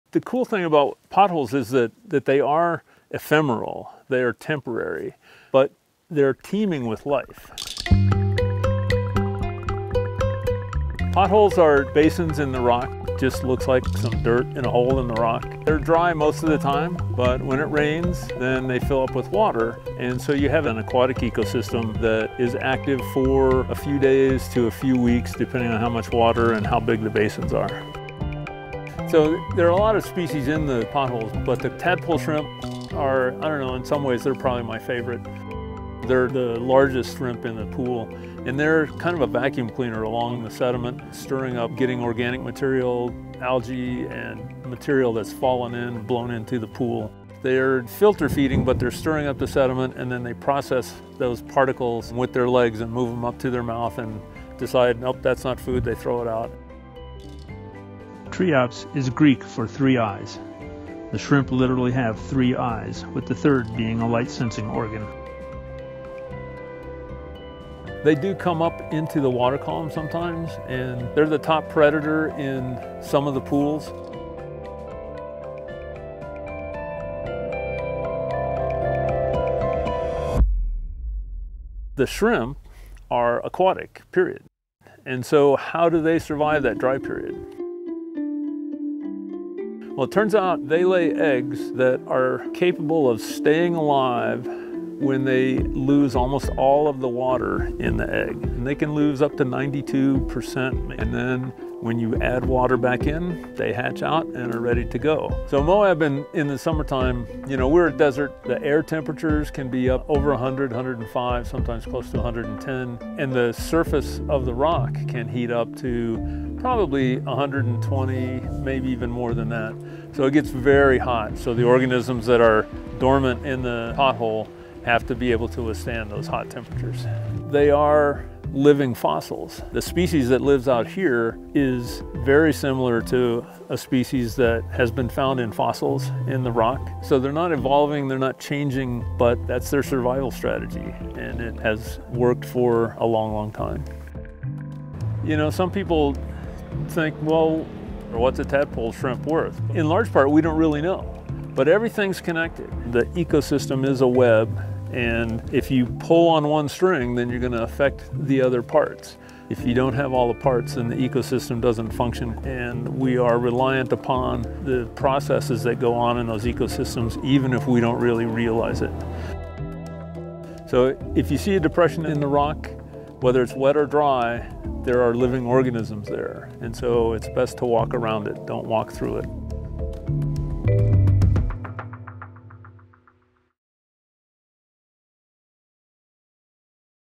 Moab's Amazing Tadpole Shrimp with Tim Graham
Did you know that Moab has three-eyed shrimp that hatch when it rains? The mysterious Tadpole Shrimp, AKA Triops, is one of the most interesting science stories in Moab’s high desert landscape. They hatch out on the bare rock during the warm rains of summer. Special thanks to retired USGS Scientist Tim Graham for sharing his insights on these beastly shrimp!

Produced for Moab Area Travel Council by Finley Holiday Productions

DM-991
3/2023